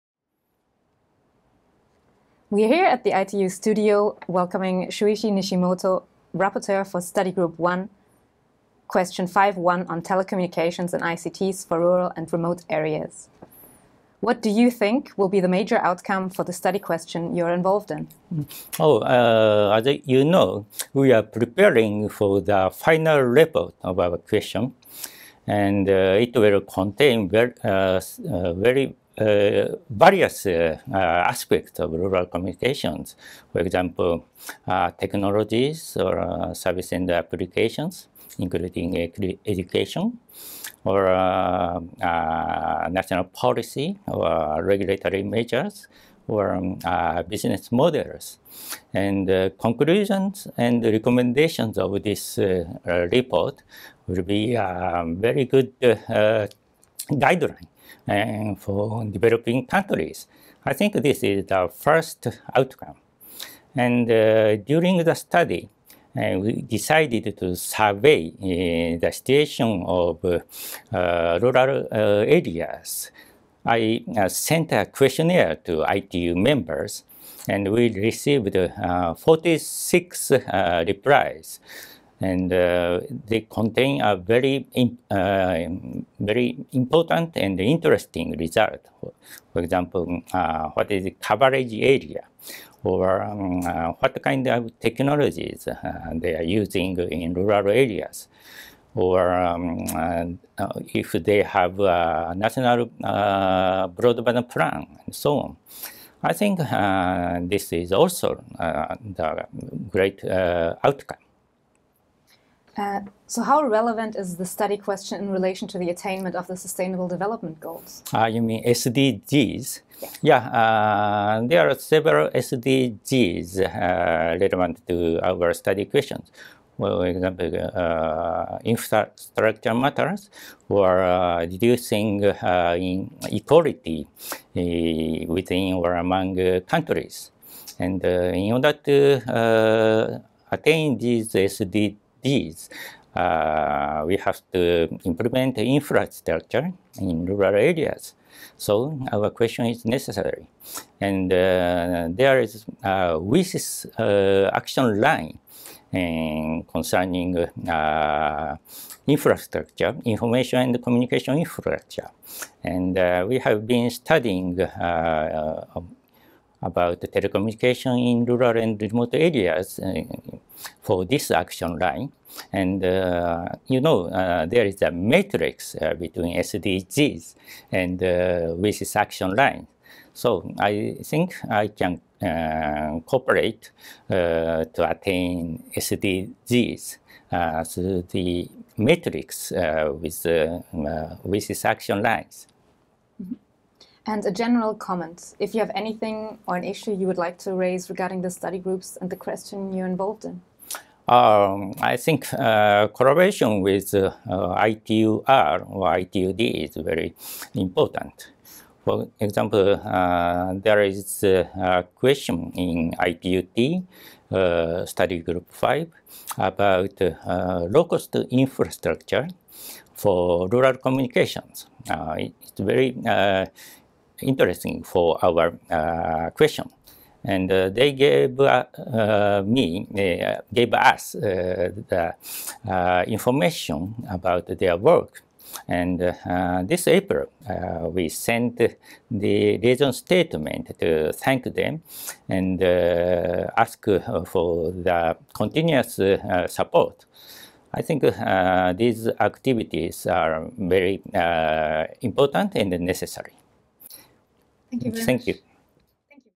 Shuichi Nishimoto, Rapporteur, ITU-D Study Group 1 Question 5/1
Interview with Shuichi Nishimoto, Rapporteur, ITU-D Study Group 1 Question 5/1: Telecommunications/ICTs for rural and remote areas.